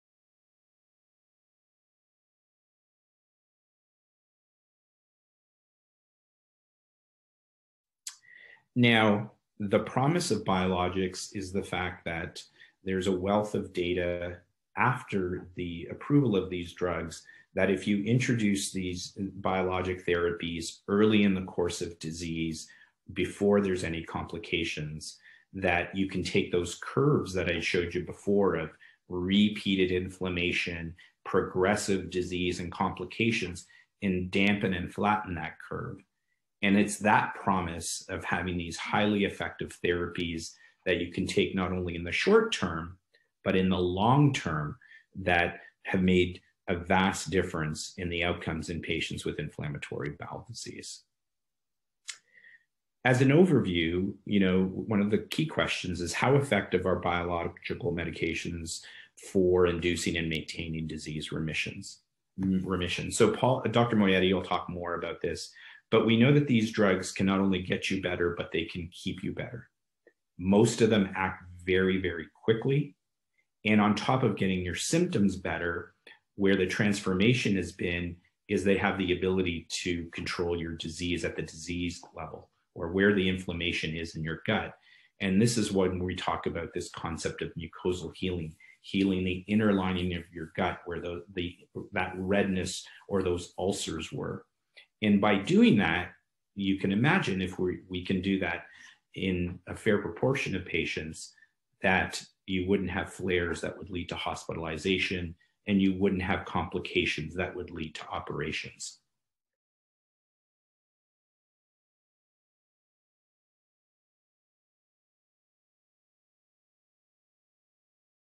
Benefits of Biologics in IBD Treatment
Watch this video to learn about the benefits of using biologics to treat inflammatory bowel disease from an expert gastroenterologist.

Speaker:
Dr. Remo Panaccione (MD, FRCPC), Professor and Gastroenterologist, Director of the Inflammatory Bowel Disease Clinic, Department of Medicine, University of Calgary.